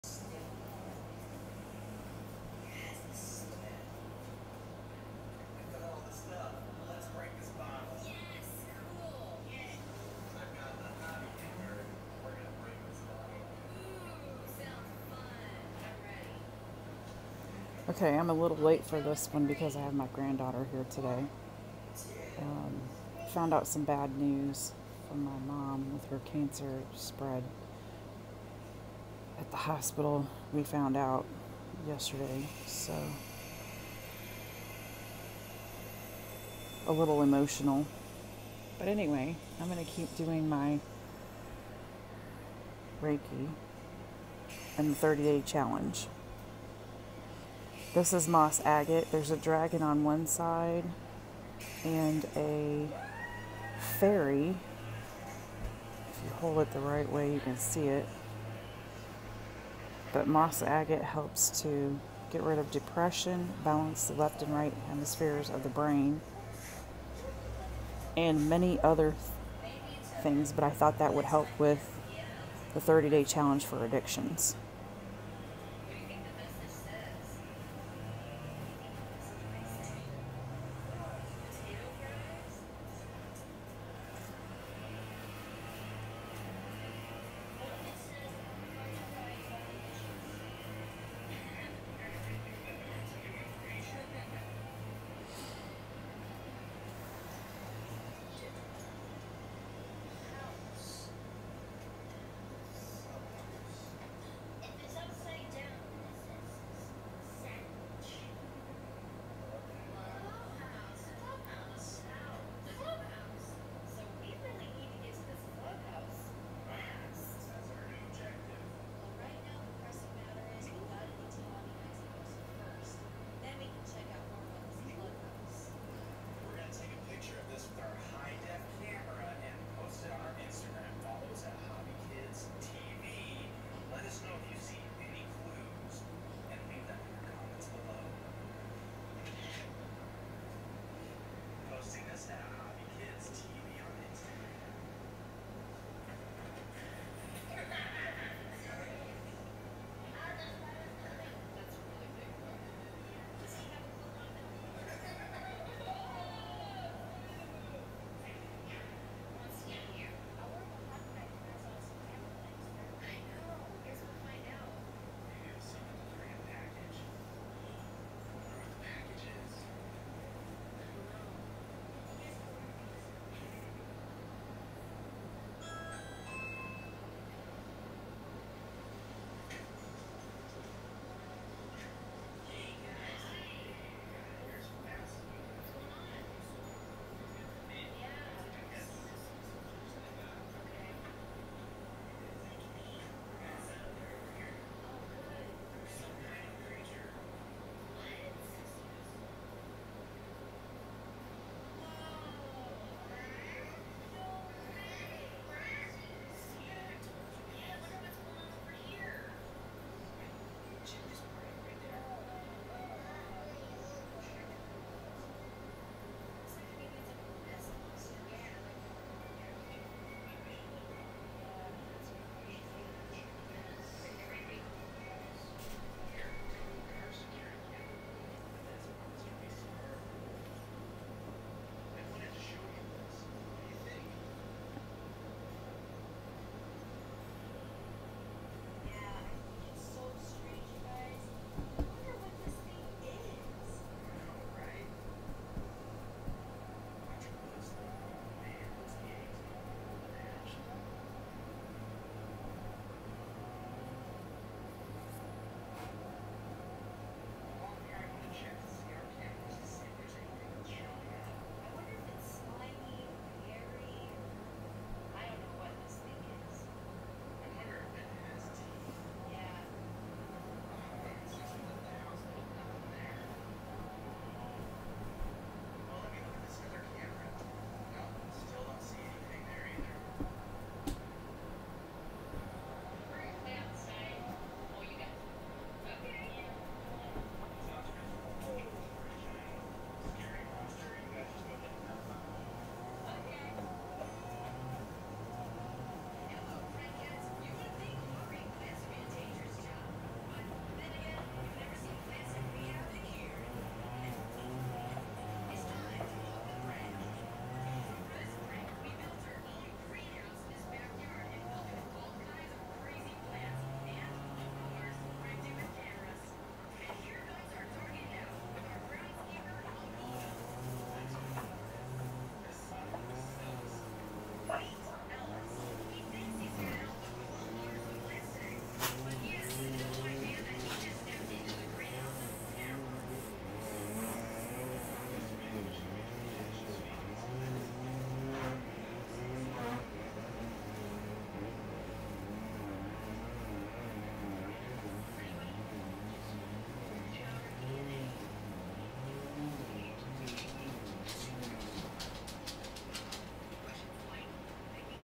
Reiki 30 day challenge and Moss Agate
Moss Agate is a quite spiritual stone that has a number of excellent qualities, including grounding excess spiritual energy, down through the base and earth chakra to Mother Gaia.

This green stone may assist you to correct imbalances between the two sides of the brain, and it resonates with a lovely heart based vibration, that most of you will find useful to have in your life. Also helps with depression and anxiety. I thought this would be a great stone for the 30 day challenge for Addictions.
Please don’t drive while watching my videos!
As usual I am not a Dr. & this is not a replacement for the help they provide this is an energy therapy to be used in conjunction with the help a Dr. or Psychologist.